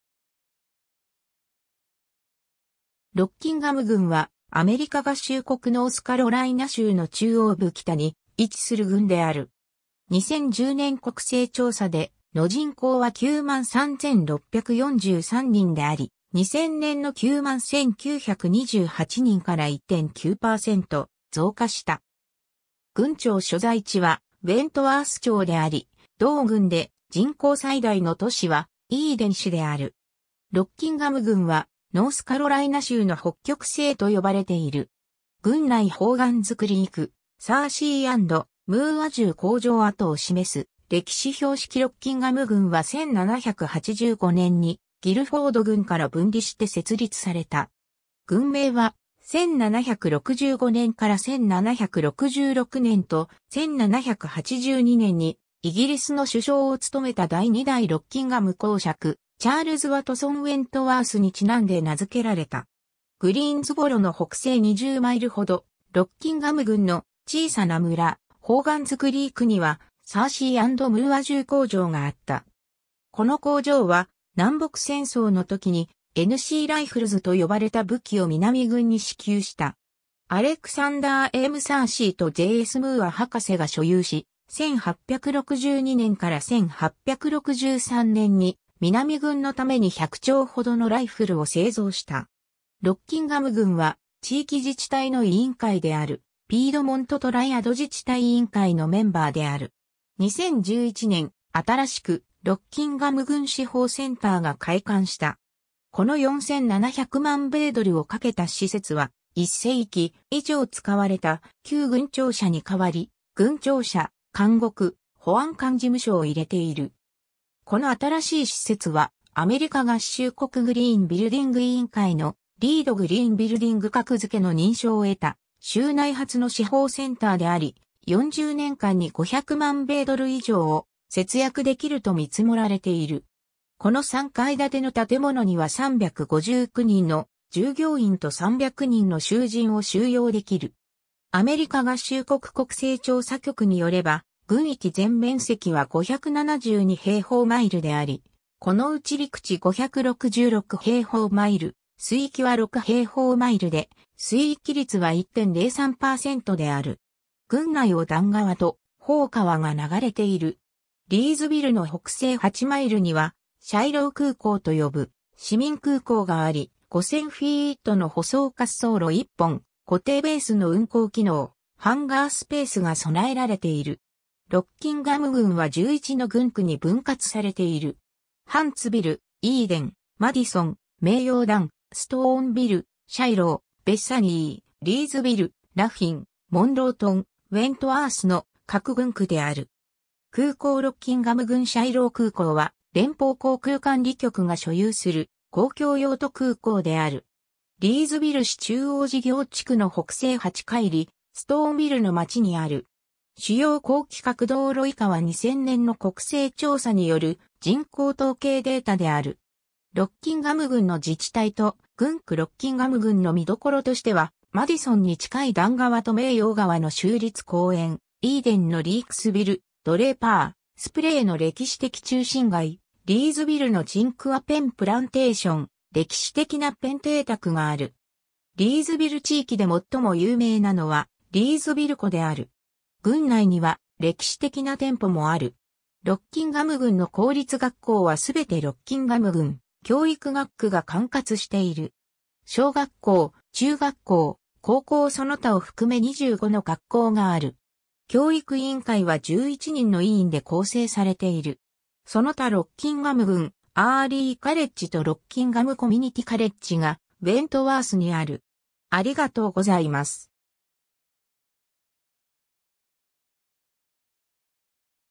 ロッキンガム郡 (ノースカロライナ州)
ロッキンガム郡（英: Rockingham County）は、アメリカ合衆国ノースカロライナ州の中央部北に位置する郡である。
2010年国勢調査での人口は93,643人であり、2000年の91,928人から1.9%増加した。
郡庁所在地はウェントワース町（人口2,807人）であり、同郡で人口最大の都市はイーデン市（人口15,527人）である。
ロッキンガム郡は「ノースカロライナ州の北極星」と呼ばれている。
 郡内ホーガンズクリーク、サーシー&ムーア銃工場跡を示す歴史標識 ロッキンガム郡は1785年にギルフォード郡から分離して設立された。
郡名は、1765年から1766年と、1782年にイギリスの首相を務めた第2代ロッキンガム侯爵チャールズ・ワトソン＝ウェントワースに因んで名付けられた。
 グリーンズボロの北西20マイル (32 km) ほど、ロッキンガム郡の小さな村ホーガンズクリークにはサーシー&ムーア銃工場があった。
この工場は南北戦争の時に「N.C.ライフルズ」と呼ばれた武器を南軍に支給した。
アレクサンダー・M・サーシーとJ・S・ムーア博士が所有し、1862年から1863年に南軍のために100挺ほどのライフルを製造した。
 ロッキンガム郡は、地域自治体の委員会であるピードモント・トライアド自治体委員会のメンバーである。
 2011年、新しくロッキンガム郡司法センターが開館した。
この4,700万米ドルを掛けた施設は、1世紀以上使われた旧郡庁舎に代わり、郡庁舎、監獄、保安官事務所を入れている。
この新しい施設はアメリカ合衆国グリーン・ビルディング委員会のリード・グリーン・ビルディング格付けの認証を得た州内初の司法センターであり、40年間に500万米ドル以上を節約できると見積もられている。
この3階建ての建物には359人の従業員と300人の囚人を収容できる。
 郡内をダン川とホー川が流れている。
 リーズビルの北西8マイル (13 km) にはシャイロー空港と呼ぶ市民空港があり、5,000フィート (1,500 m) の舗装滑走路1本、固定ベースの運行機能、ハンガースペースが備えられている。
 ロッキンガム郡は11の郡区に分割されている。
ハンツビル、イーデン、マディソン、メイヨーダン、ストーンビル、シャイロー、ベサニー、リーズビル、ラフィン、モンロートン、ウェントワースの各郡区である。
 空港 ロッキンガム郡シャイロー空港は、連邦航空管理局が所有する公共用途空港である。
リーズビル市中央事業地区の北西8海里 (15 km)、ストーンビルの町にある。
 主要高規格道路 以下は2000年の国勢調査による人口統計データである。
 ロッキンガム郡の自治体と郡区 ロッキンガム郡の見どころとしては、マディソンに近いダン川とメイヨー川の州立公園、イーデンのリークスビル、奴隷パー、スプレーの歴史的中心街、リーズビルのチンクァ・ペン・プランテーション、歴史的なペン邸宅がある。
リーズビル地域で最も有名なのはリーズビル湖である。
郡内には歴史的な店舗もある。
 ロッキンガム郡の公立学校は全てロッキンガム郡教育学区が管轄している。
小学校、中学校、高校その他を含め25の学校がある。
教育委員会は11人の委員で構成されている。
その他ロッキンガム郡アーリーカレッジとロッキンガム・コミュニティカレッジがウェントワースにある。